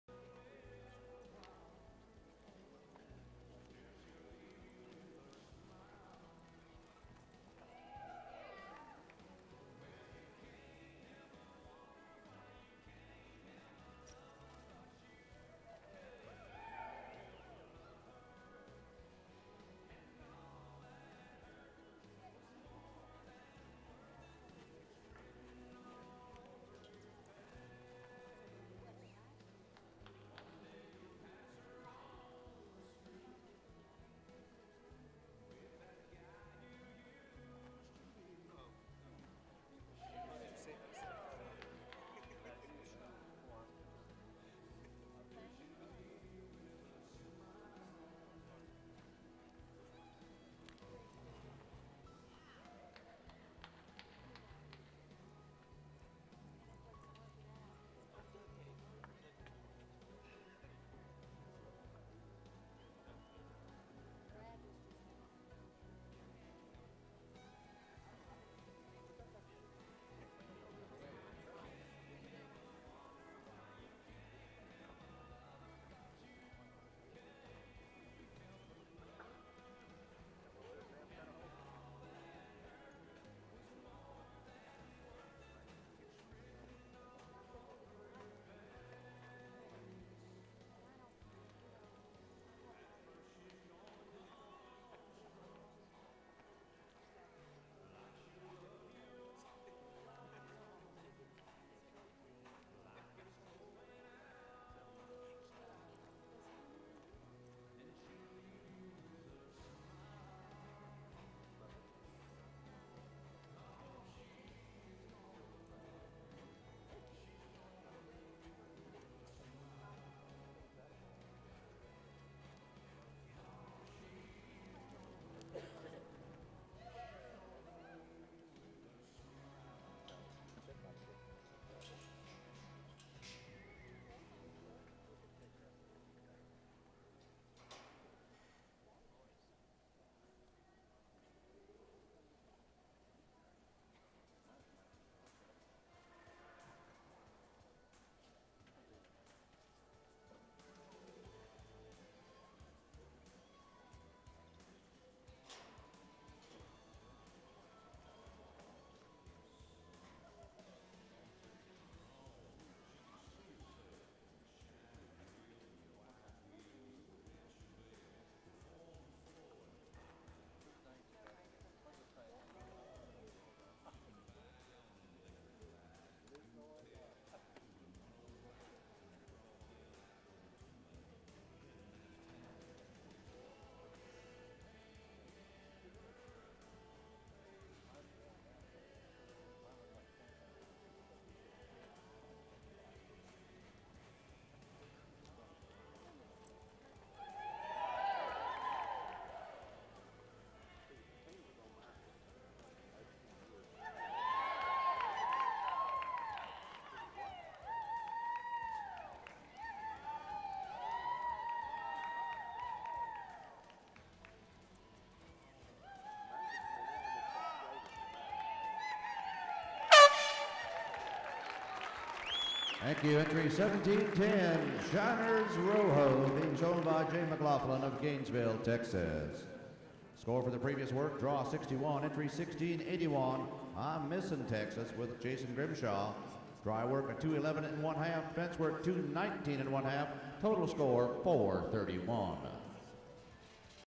2009 AQHA WORLD SHOW - SHINERS ROJO - JR. WCH
At the 2009 AQHA World Show, Shiners Rojo competes in the Junior Working Cow Horse preliminaries. Sire: Shining Spark - Dam: Little Lena Lady.